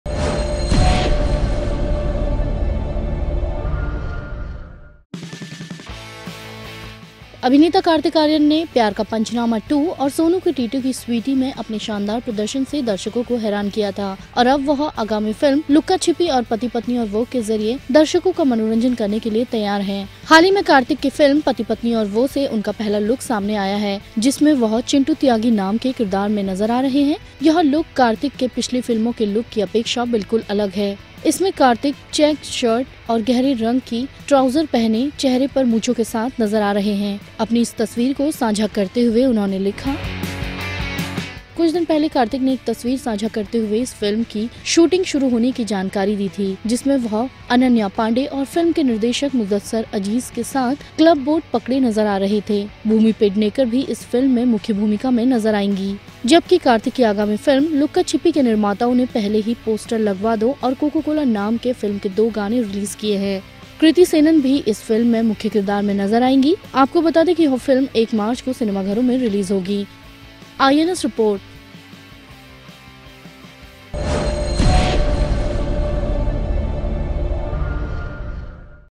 ' Pati, Patni aur Woh ' में साधारण लुक में नजर आयेगें Kartik
अभिनेता कार्तिक आर्यन  ने ' प्यार का पंचनामा 2 ' और ' सोनू के टीटू की स्वीटी ' में अपने शानदार प्रर्दशन से दर्शकों को हैरान किया था।  और अब वह आगामी फिल्म 'लुक्का - छिप्पी' और '  पति-पत्नी और वो'   के जरिये दर्शकों का मनोरंजन करने के लिये तैयार हैं।